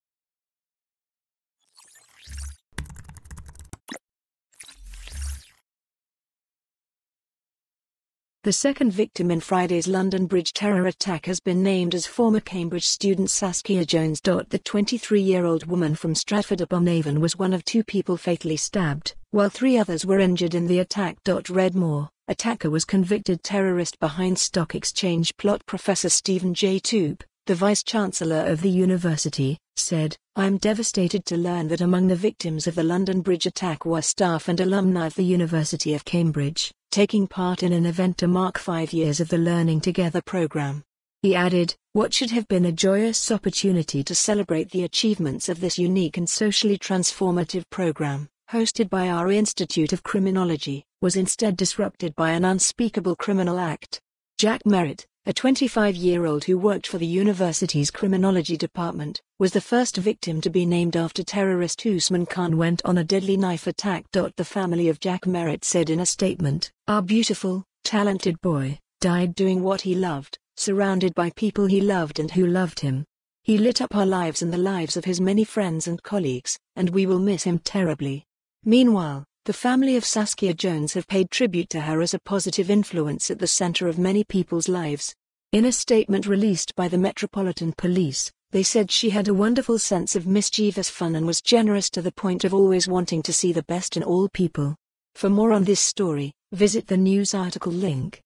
London Bridge attack: Second victim named
The second victim in

Friday's London Bridge terror attack has been named as former

Cambridge student Saskia Jones.The 23-year-old woman from Stratford-upon-Avon was one of two people fatally stabbed, while three others were injured in the attack.Read more: Attacker was convicted terrorist behind stock exchange plotProfessor Stephen J Toope, the vice-chancellor of the university, said: "I am devastated to learn that among the victims of the London Bridge attack were staff and alumni of the University of Cambridge, taking part in an event to mark five years of the Learning Together programme."He added: "What should have been a joyous opportunity to celebrate the achievements of this unique and socially transformative programme, hosted by our Institute of Criminology, was instead disrupted by an unspeakable criminal act."Jack Merritt, a 25-year-old who worked for the university's criminology department, was the first victim to be named after terrorist Usman Khan went on a deadly knife attack.The family of Jack Merritt said in a statement: "Our beautiful, talented boy, died doing what he loved, surrounded by people he loved and who loved him. He lit up our lives and the lives of his many friends and colleagues, and we will miss him terribly."Meanwhile, the family of Saskia Jones have paid tribute to her as a "positive influence at the centre of many people's lives".In a statement released by the Metropolitan Police, they said "she had a wonderful sense of mischievous fun and was generous to the point of always wanting to see the best in all people".